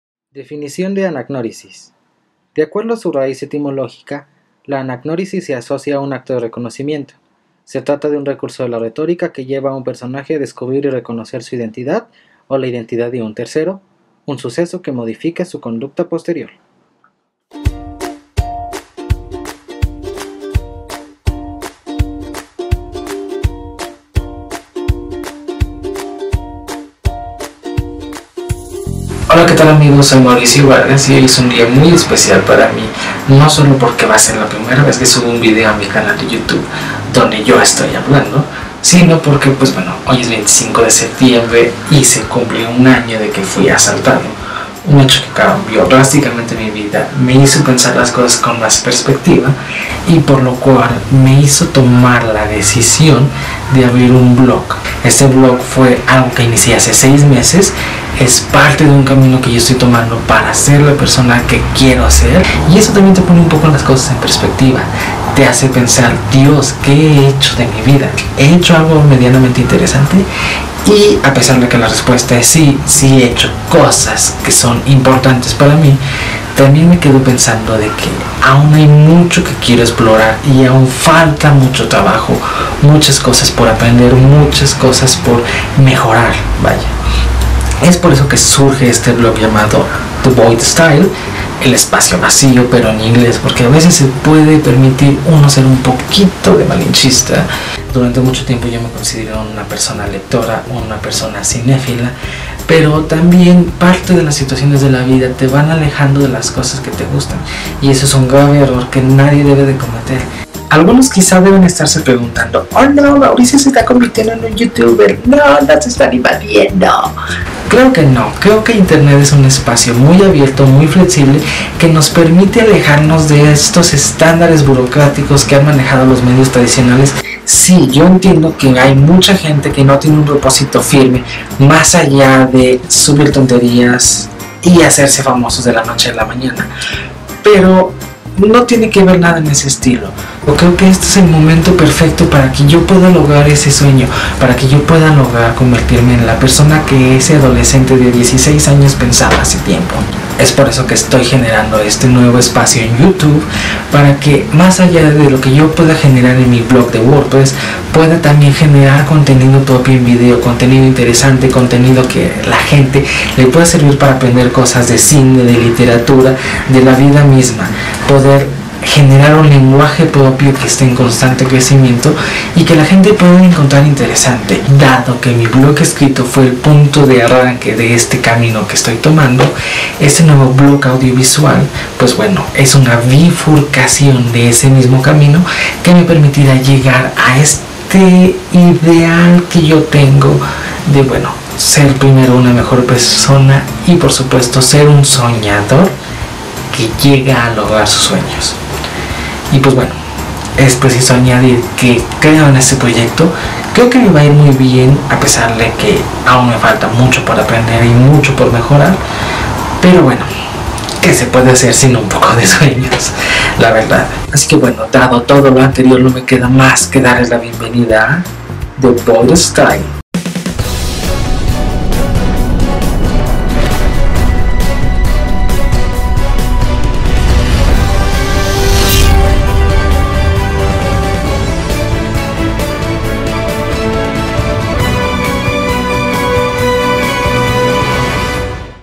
Presentando el Vlog | The Void Stile
Presento a The Void Stile, el espacio vacio del internet ¡ahora en formato de Vlog!

Este es el primer video para este canal, el cual estará enfocado principalmente a Cine, Literatura y cultura general.

¡El viaje por el universo comienza aquí!